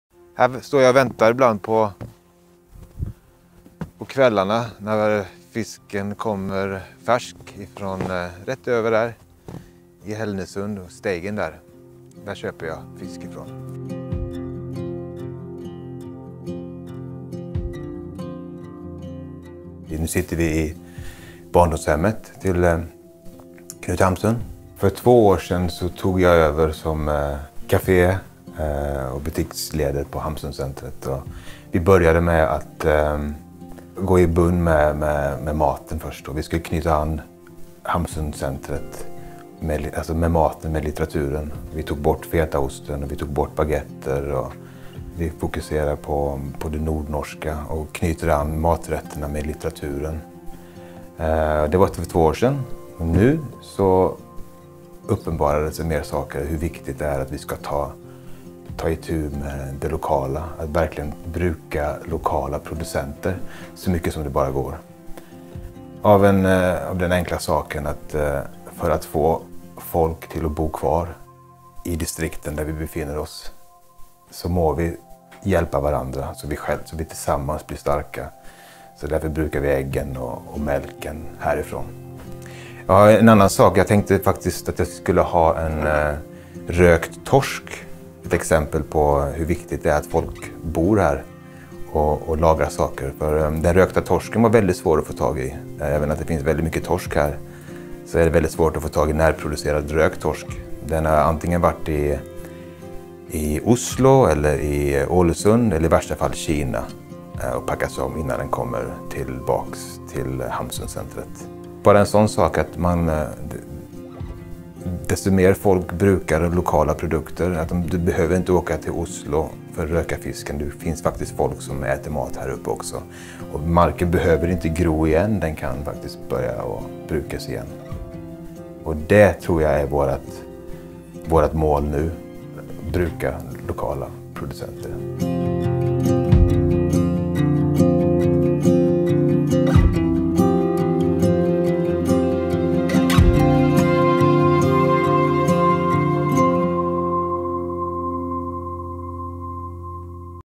Røkt torsk fra Kina – og behovet for norske økoløft
I kjøkkenet på Hamsunsenteret står Alfred Liabäck og forbereder dagens servering. Bygg og havre til grøt, nystekte fiskekaker og vafler med nyrørte bær og geitost. For to år siden, da han tok over driften av Kafé Sult, var serveringstilbudet likt det du får på mange norske kaféer – bagetter, salater og en hel del fetaost. God mat, joda, men uten forankring i Hamarøy og regionen den serveres i, og uten tilknytning til historiene som fortelles. I dag er visjonen om kafédrift som del av et lokalt, bærekraftig kretsløp klar, men veien dit er ikke uten hindringer.

Samtidig, i Økologisk Norges lokaler i Oslo, sitter Markus Brun Hustad. Han er prosjektleder i Landbrukets Økoløft, prosjektet som skal bidra til økt produksjon av økologiske landbruksprodukter i Norge, og som Debio og DebioInfo også er en del av. Skal Alfred og Hamsunsenteret servere økologiske måltider, må de også kunne kjøpe råvarene fra lokale leverandører, noe som det i dag er temmelig vanskelig å få til. Hvordan kan vi få økt antallet økologiske produsenter i Norge? Og hvordan kan en kafé i Salten bidra til det?

Produsert av Heia Folk!